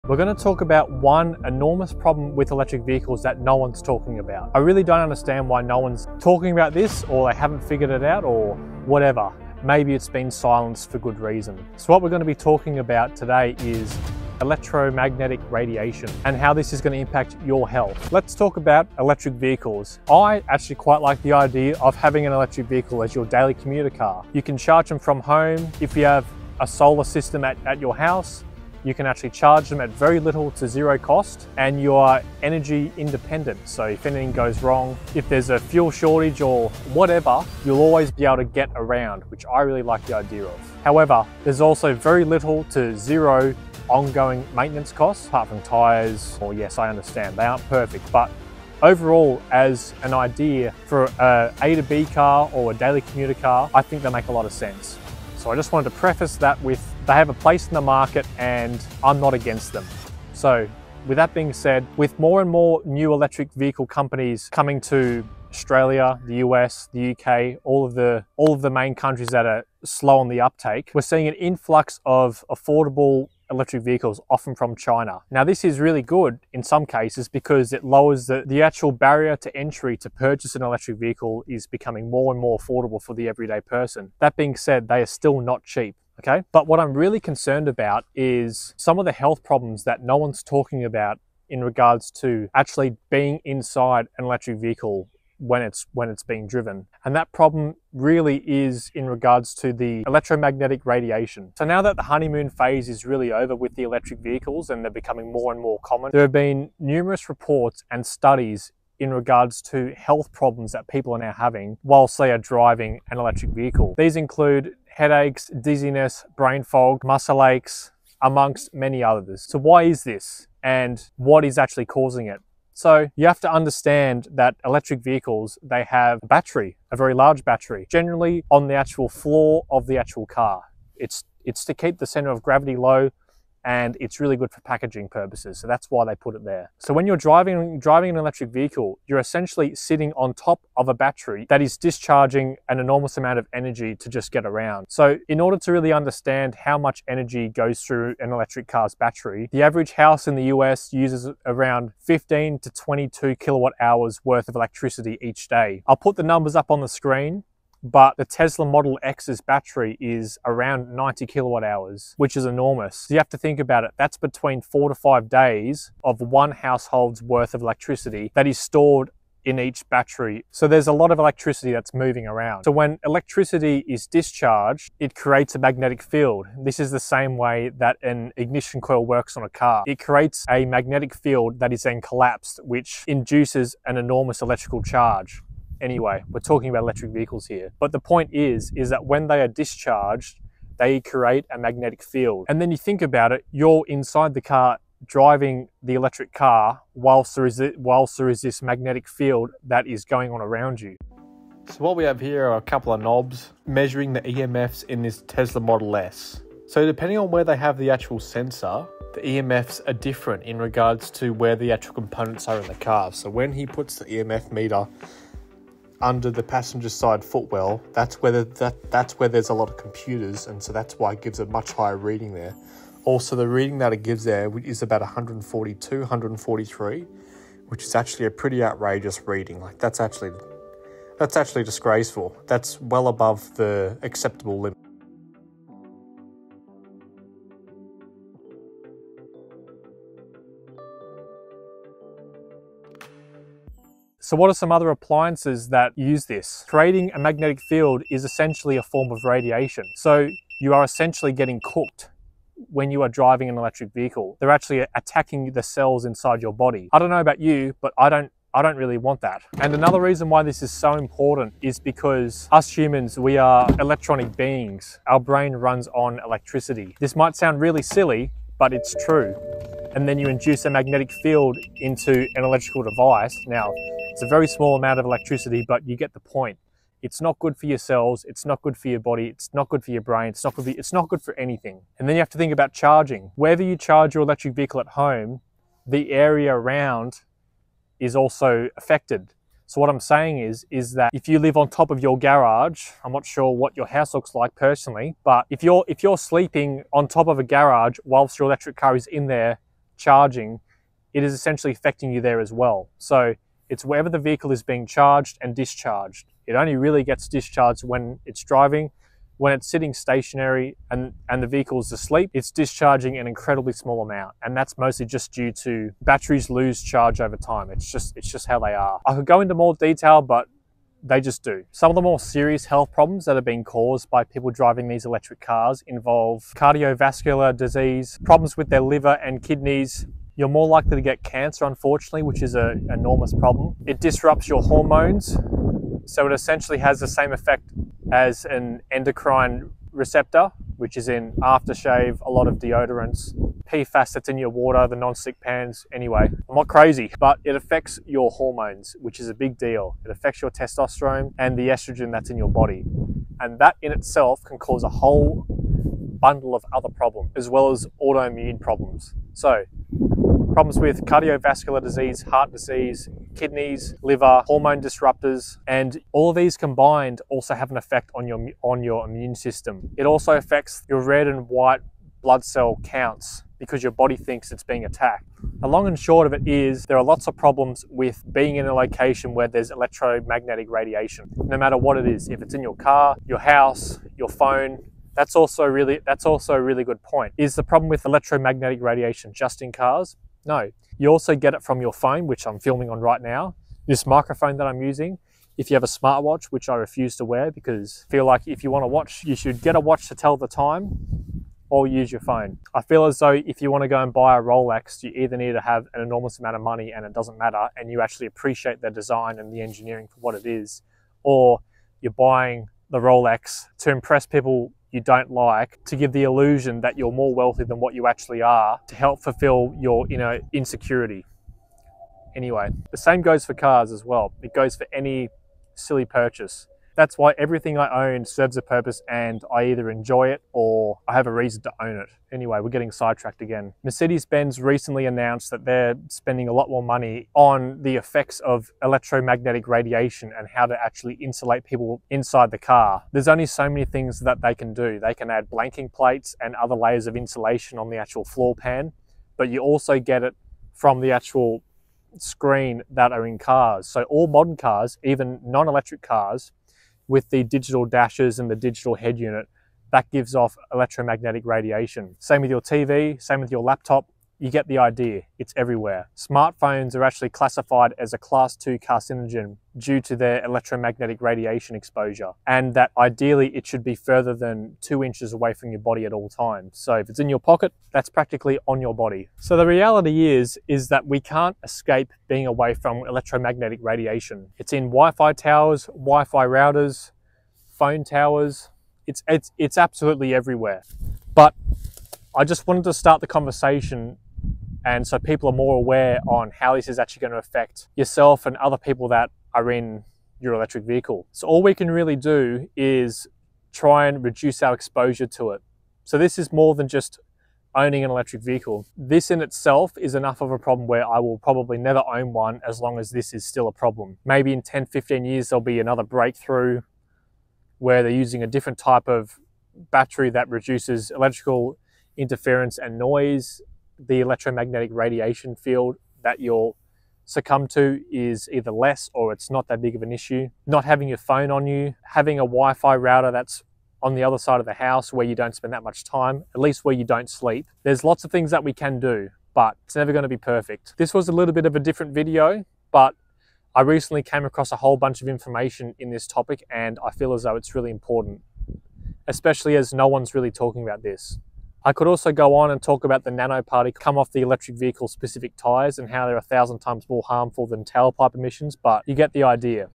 Radiation in Electric Cars
In this video, we’ll explore:

What Electromagnetic Radiation Is: Understanding the basics of electromagnetic fields and their sources.

Sources in Electric Cars: A detailed look at where electromagnetic radiation comes from in electric vehicles, including batteries, electric motors, and charging systems.

Health Implications: What the science says about the potential health effects of electromagnetic radiation from electric cars.

Real-World Impact: Insights from experts and real-world data on the levels of electromagnetic radiation in electric cars compared to other common sources.

Whether you’re an EV enthusiast, a potential buyer, or just curious about how electric cars operate, this video provides essential information and clears up common misconceptions. 🔔👍